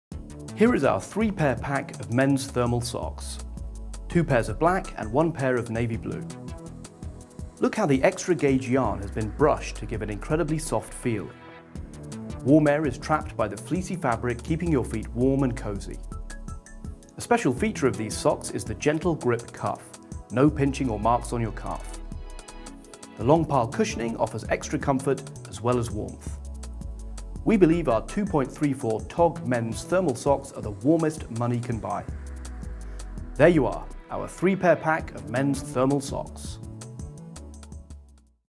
Thermal Socks - Pack of 3
When it comes to thermal socks we think these are the warmest money can buy. They have an incredibly high tog rating of 2.34 keeping your feet really well insulated. The specially developed extra heavy bulk acrylic yarn has been brushed to give an incredibly soft feel, while trapping warm air close to the skin - the result, much warmer feet for much longer period.
And the long pile cushioning, means these socks will not only help keep feet cosy, they give extra support too.

91% Acrylic, 5% Nylon 3% Polyester 1% Elastane
Gentle grip cuff
Normal seamed toes
Provided in a pack of 3 - 2 Black and 1 Navy
Ladies Size 3-8